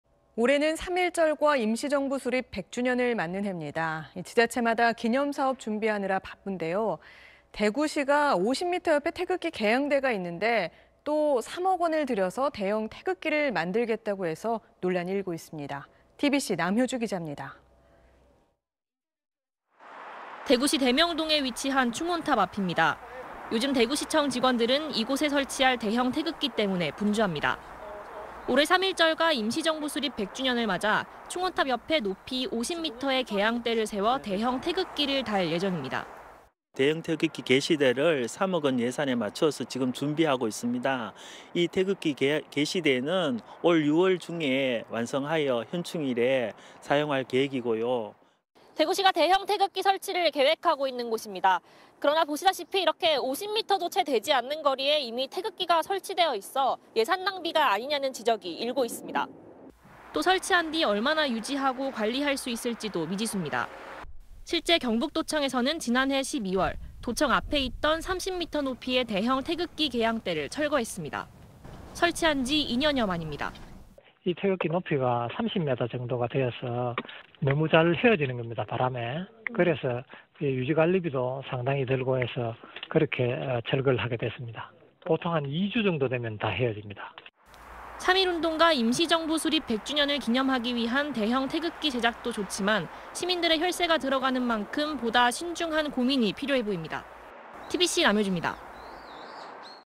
50m 옆에 태극기 있는데…'3억 대형 태극기' 설치 논란 / SBS
올해는 3.1절과 임시정부 수립 100주년을 맞는 해입니다. 지자체마다 기념사업 준비하느라 바쁜데요, 대구시가 50m 옆에 태극기 게양대가 있는데 또 3억 원을 들여서 대형 태극기를 만들겠다고 해서 논란이 일고 있습니다.
TBC 남효주 기자입니다.